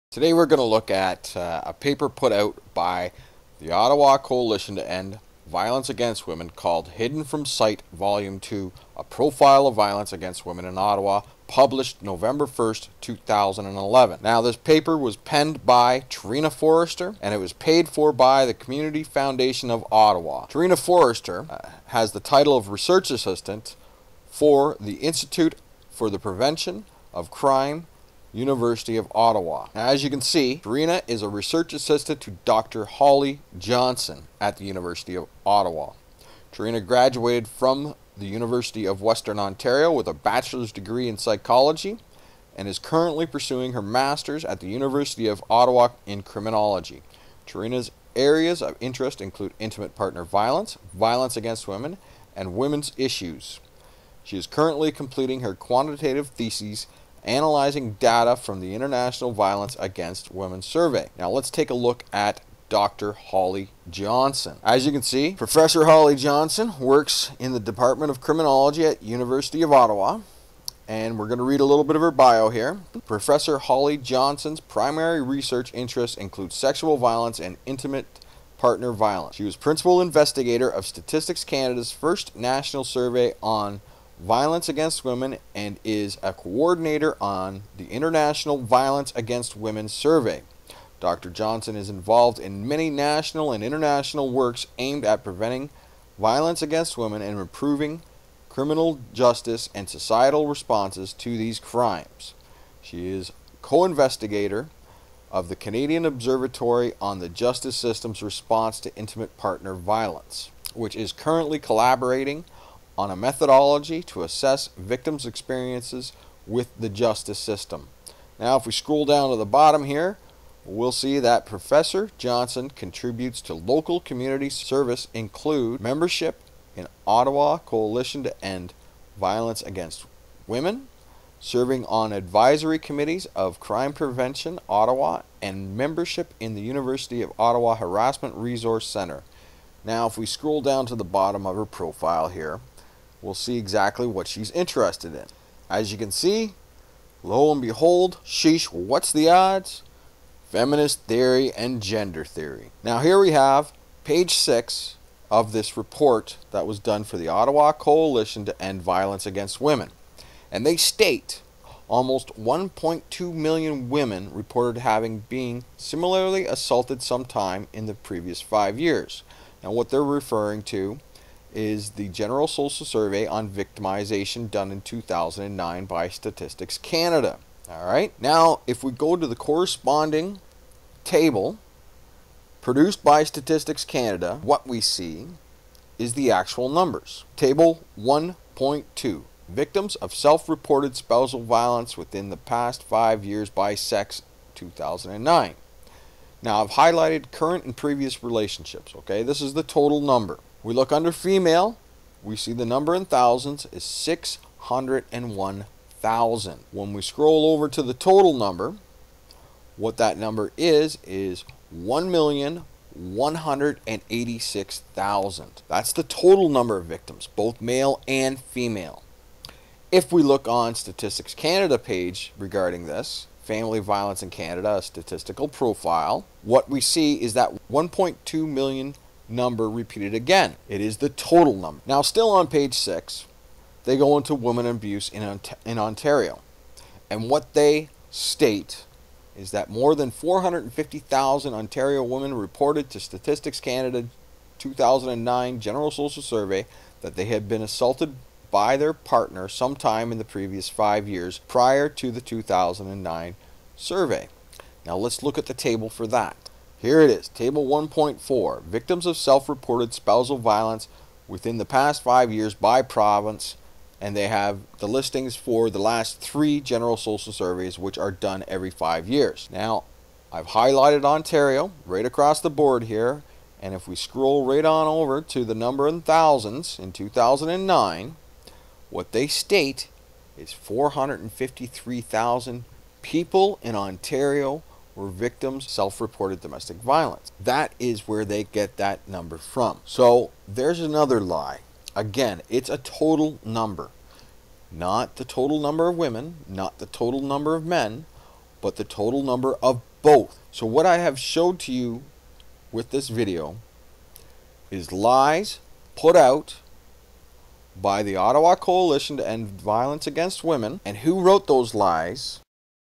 Domestic Violence Lies From Ottawa
Many thanks to scatmaster for pointing this article out.
Feel free to mirror

Article in the Ottawa Sun which lead me to these lying misandric cretins.